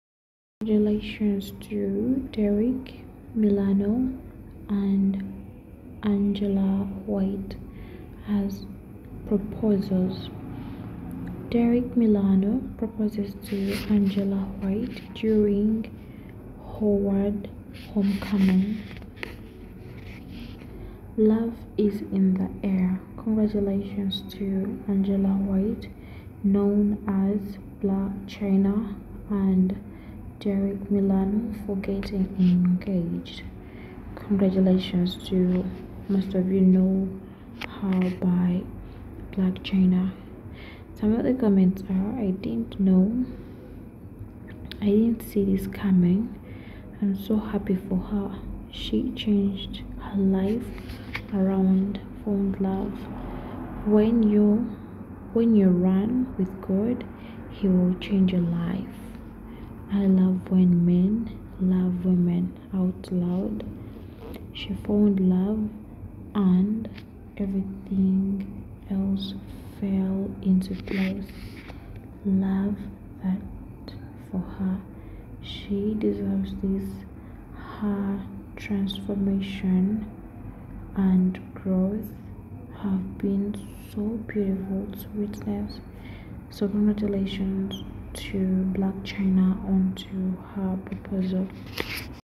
BLAC CHYNA ENGAGED TO BOYFRIEND DERRICK MILANO /Derrick Milano Proposes To Angela White, VIDEO
Derrick Milano Proposes To Angela White During Howard Homecoming